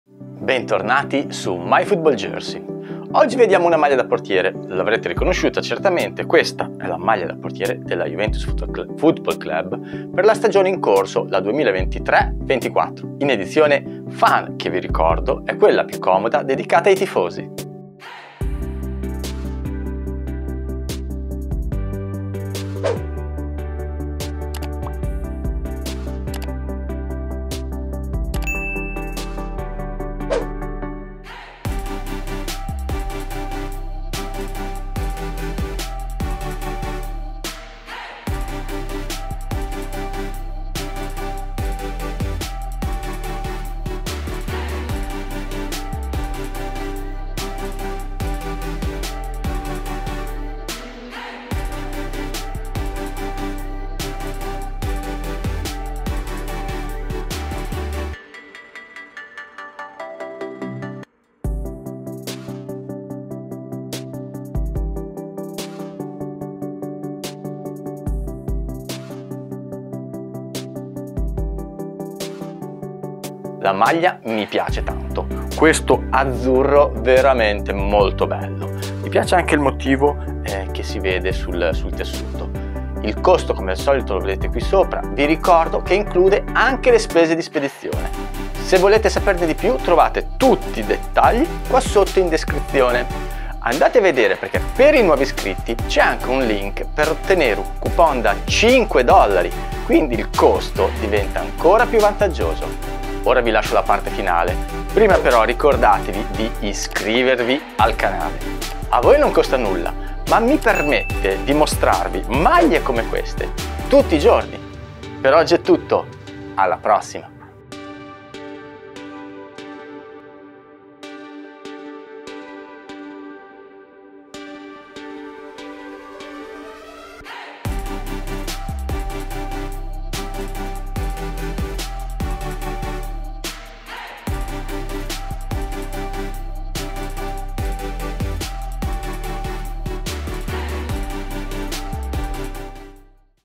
Juventus Goalkeaper Jersey 23/24 (Minejerseys) Fan Version Unboxing Review
Discover the Juventus Goalkeaper Jersey 23/24 (Fan Version)
If for some reason the link for the Juventus Goalkeaper Jersey 23/24 doesn't work, write a comment about it, I will reply as soon as possible.

The Juventus Goalkeaper Jersey 23/24 (Fan Version) costs only 24,99 $! ** Shipping INCLUDED **

Find out the measurements of the Juventus Goalkeaper Jersey 23/24 (Fan Version).
Size: Medium
Height: 175 cm (5 ft 9 in)
Weight: 70 Kg (154 lbs)

Timecodes
00:00 Overview
00:25 Details View
01:14 Pricing
01:58 Dimensions & Try On

Please watch the Juventus Goalkeaper Jersey 23/24 (Fan Version) video review in 1080p (HD)